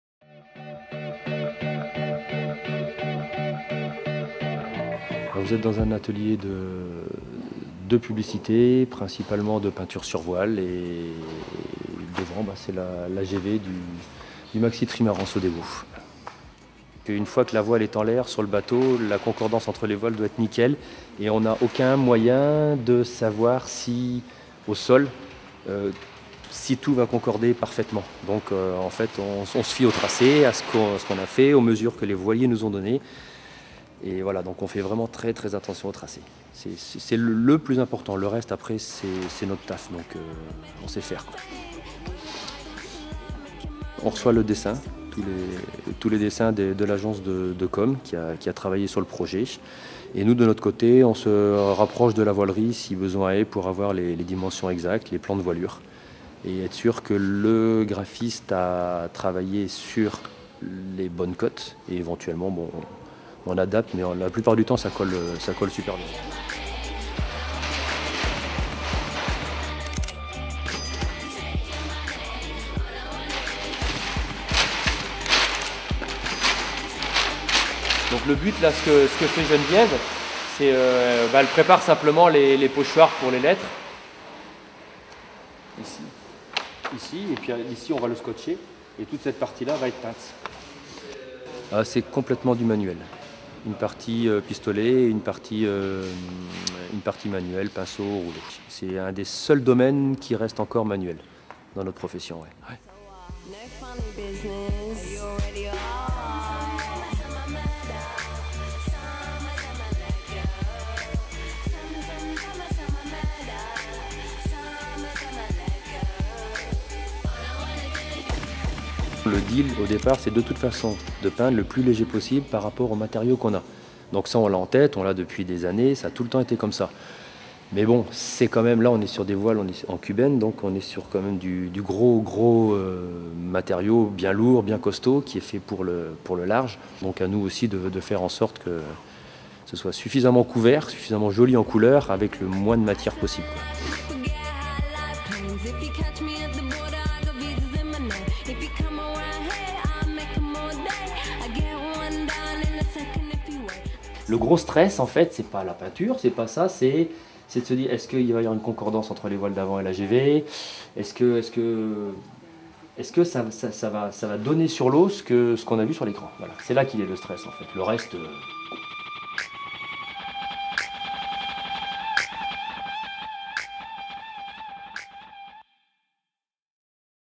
La Grande Voile de SODEB'O se fait une beauté
En parallèle du chantier, la Grande Voile du maxi trimaran SODEB'O a été confiée aux bons soins de François Callens et son équipe. A eux de faire une beauté à cette grande toile.